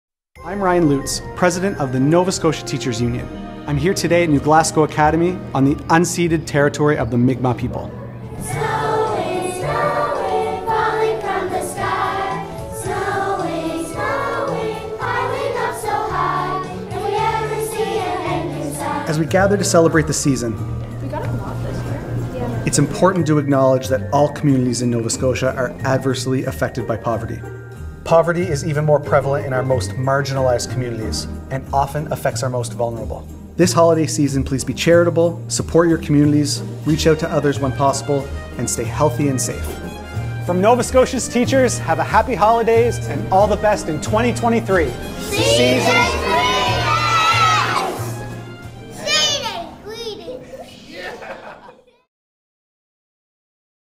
NSTU President Ryan Lutes Wishing everyone a Happy Holidays in 2022.
Hi. I'm Ryan Lutes, President of the Nova Scotia Teacher's Union. I'm here at New Glasgow Academy on the unceded territory of the Mi'kmaw people.

(singing) Snowing Snowing falling …

As we gather to celebrate the season, it's important to acknowledge that all communities in Nova Scotia are adversely affected by poverty.

This Holiday season, please be charitable, support your communities, reach out to others when possible, and to stay healthy and safe.

From Nova Scotia’s teachers, have a happy holidays and all the best in 2023
Season’s Greetings